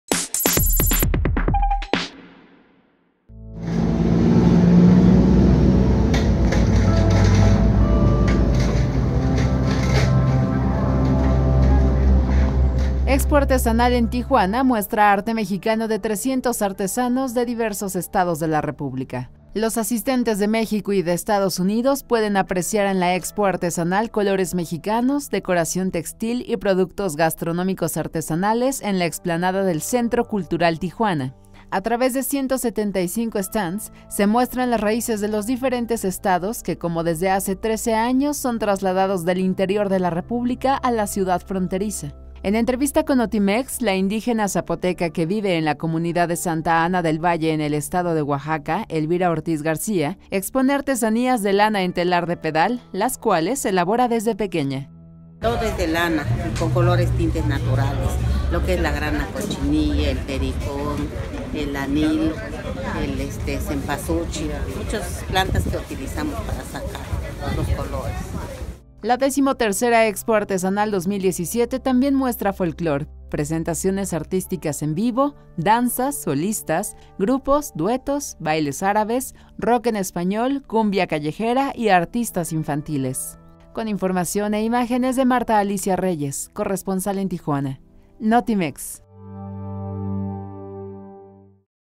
Expo Artesanal en Tijuana muestra arte mexicano de 300 artesanos
La XIII Expo Artesanal 2017 muestra el folklore y arte mexicano en un solo lugar